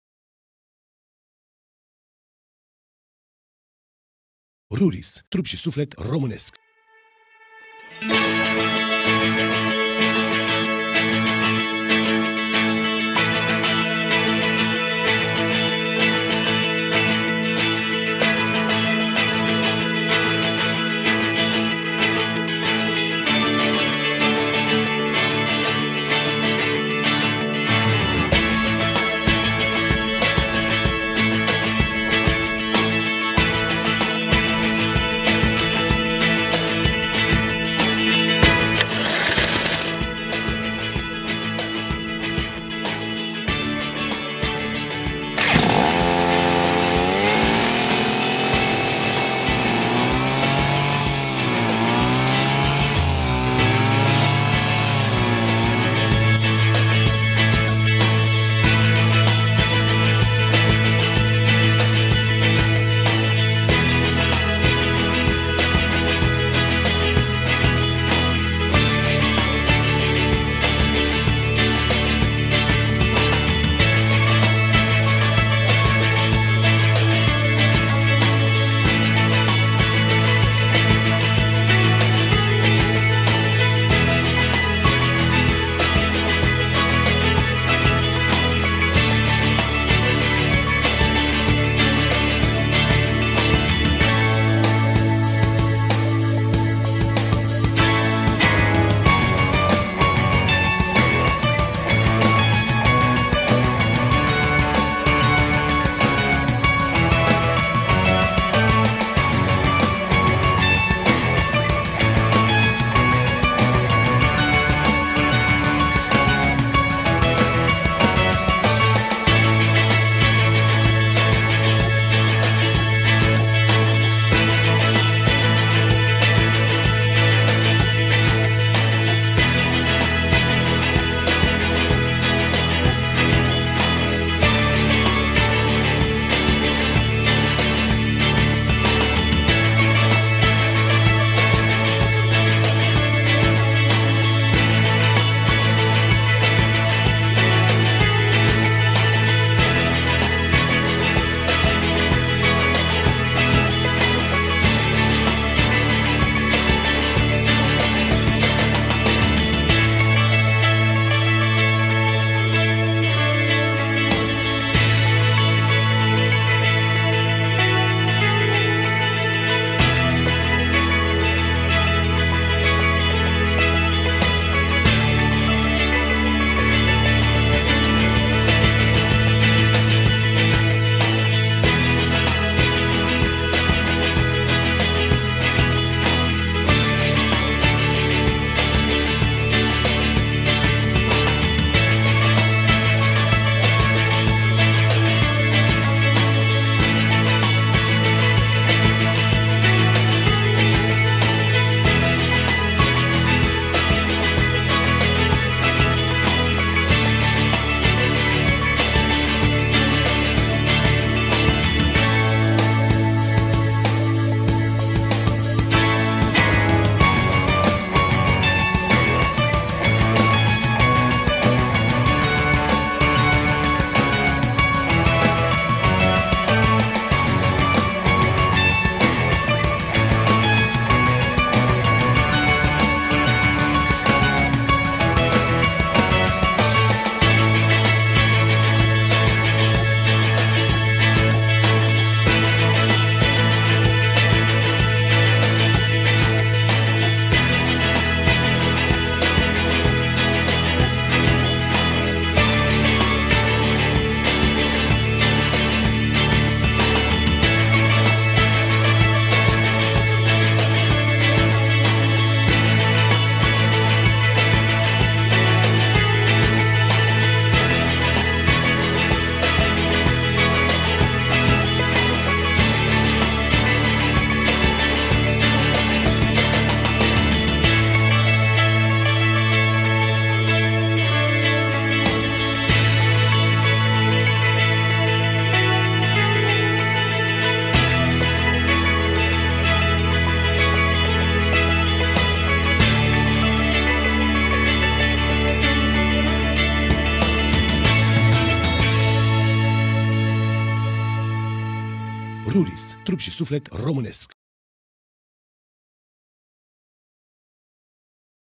Motocoasa de umăr RURIS 633ES 643ES Electric Start la lucru
Motocoasele cu pornire electrica RURIS 633ES si Ruris 643ES le gasiti aici
Date tehnice motocoasa, Ruris 643ES, 1.7 CP, pornire electrica, benzina :
Vibratii maner dreapta: 2.36 m/s²
Vibratii maner stanga: 2.37 m/s²
Diametru de taiere (opt.): 200-250 mm
Raza medie de taiere: 2100 mm
Diametru tub tija: 28 mm
Tip arbore Cu caneluri
Cutit: 3 dinti
Greutate produs: 7.6 kg.
Tip lucrare: Semi-profesional
Alimentare: Benzina
Sistem de pornire: Electric
Latimea de taiere (mm): 250
Motor: KHUMMA
Consum mediu combustibil (l/h): 0.9
Sistem de aprindere: Electronic
Cai putere (CP): 1.7
Timpi motor: 2
Capacitate cilindrica: 43 cm³
Amestec carburant (ml ulei/ litri benzina): 25/1
Producator: Ruris